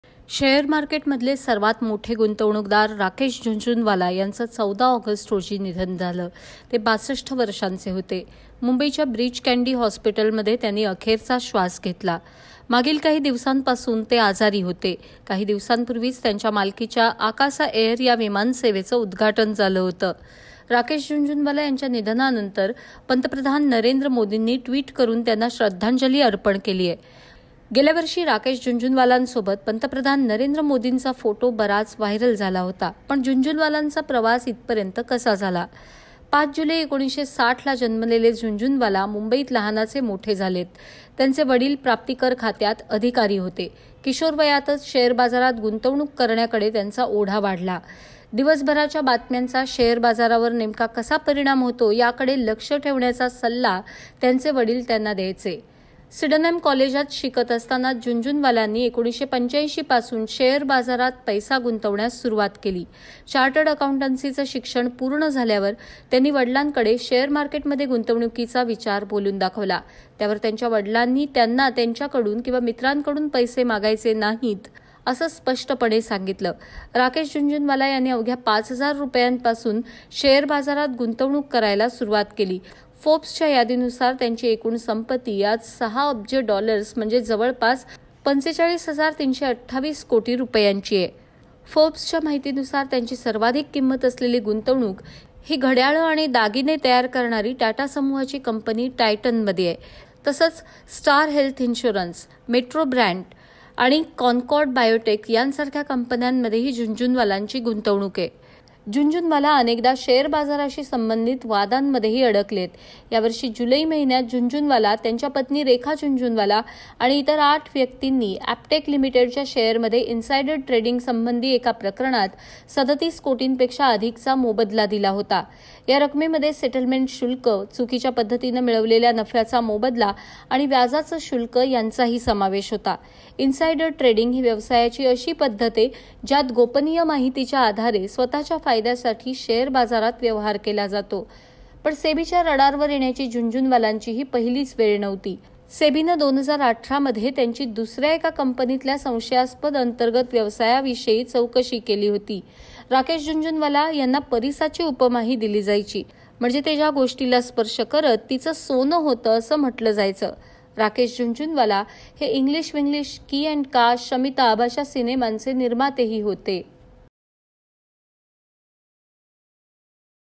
Rakesh Jhunjhunwala Death : Big Bull राकेश झुनझुनवाला कोण होते? | Share Market
भारतातले शेअर मार्केटमधील सर्वांत मोठे गुंतवणूकदार राकेश झुनझुनवाला यांचे आज (14 ऑगस्ट) निधन झाले. ते 62 वर्षांचे होते. झुनझुनवाला यांना भारताचे वॉरेन बफे म्हटलं जायचं. पण त्यांचं नाव अनेकदा वादातही सापडलं आहे.